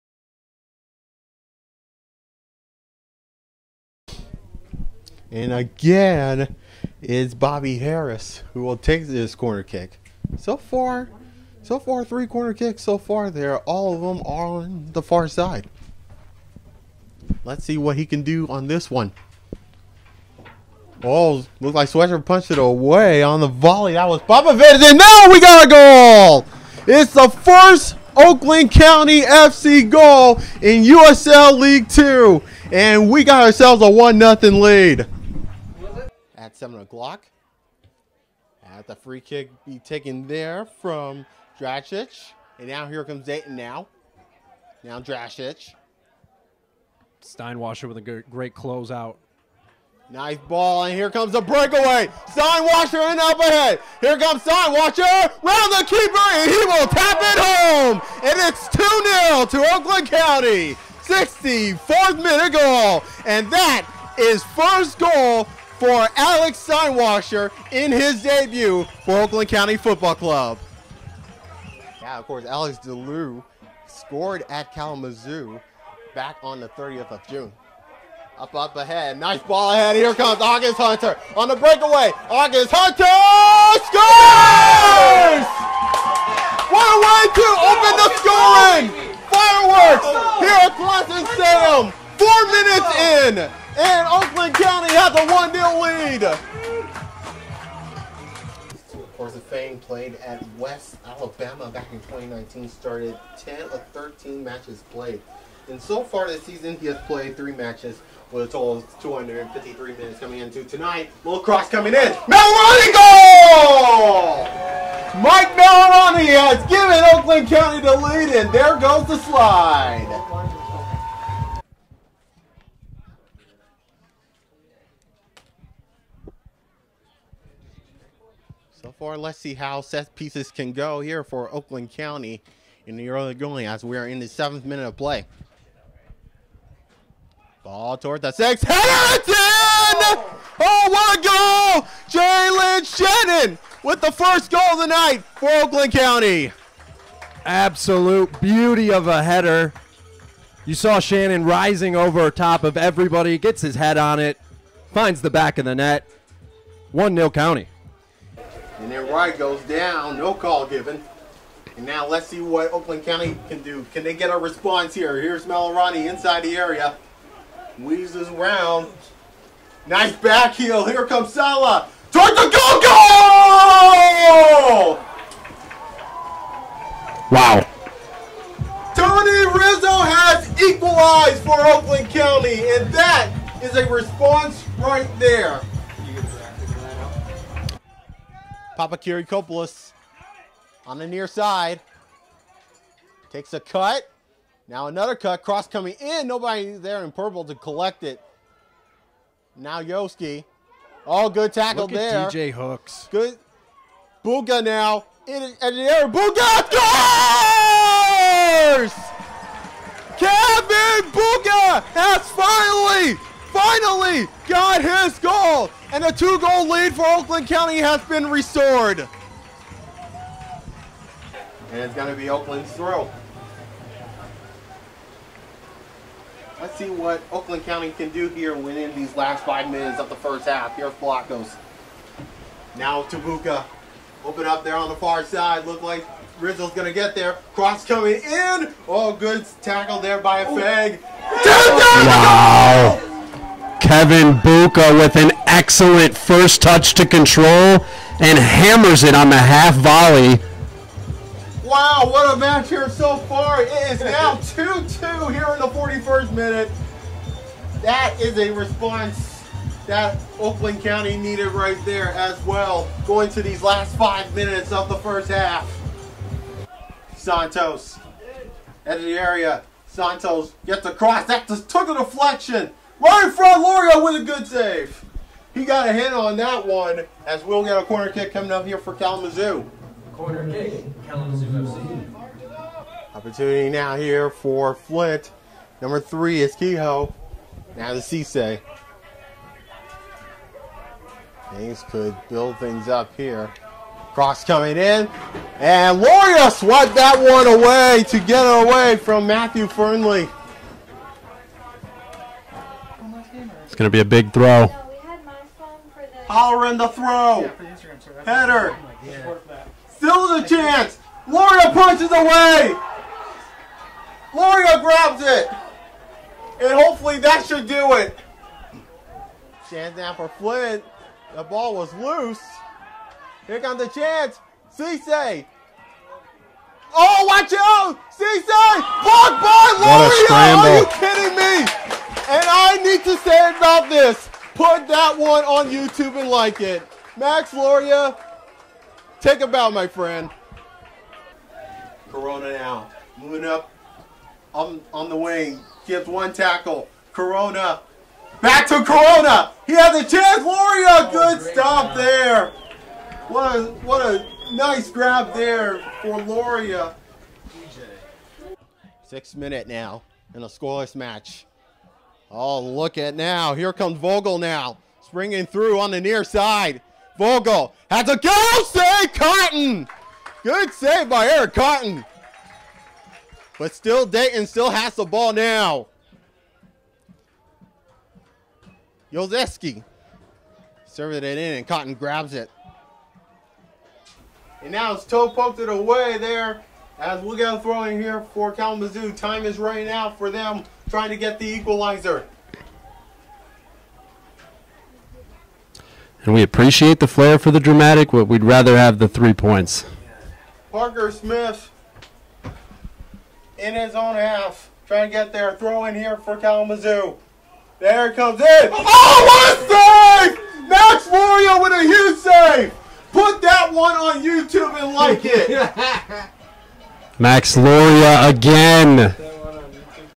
Yohn Voker Soccer Play-By-Play 2021 Demo Reel
A sample of the play-by-play commentary I did for Oakland County FC as a broadcast intern during the summer of the 2021 USL League Two season. Feedbacks are appreciated and critiques and advice for me to improve if necessary.